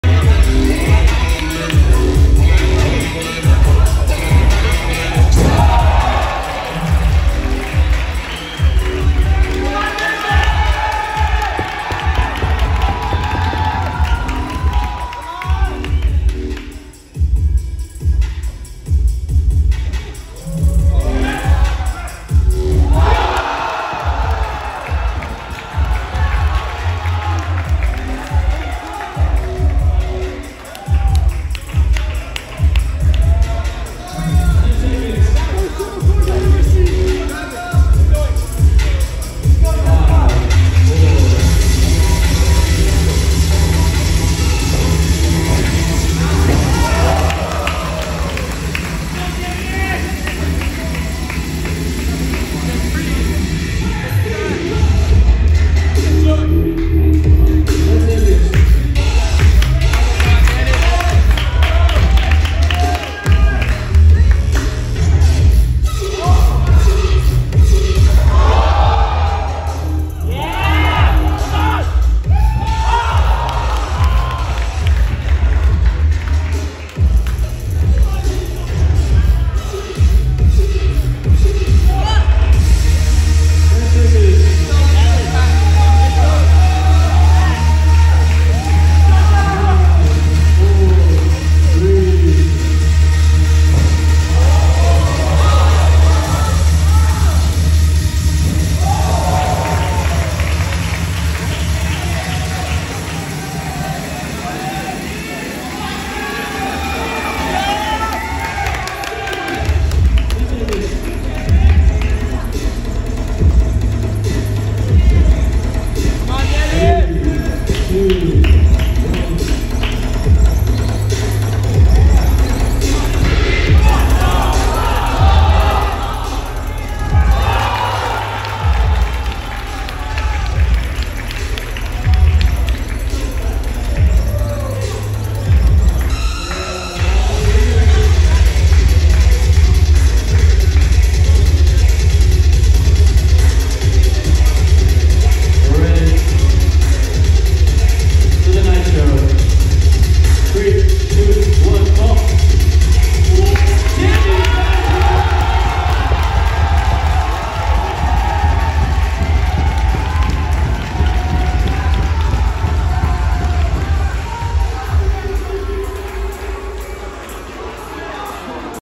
Loopkicks Gathering 2023- Top 8 Battles: Taka vs. Dandy
Featuring: Taka Enomoto (@ennuihawk), Dandy Bien (@pug_moose.lk)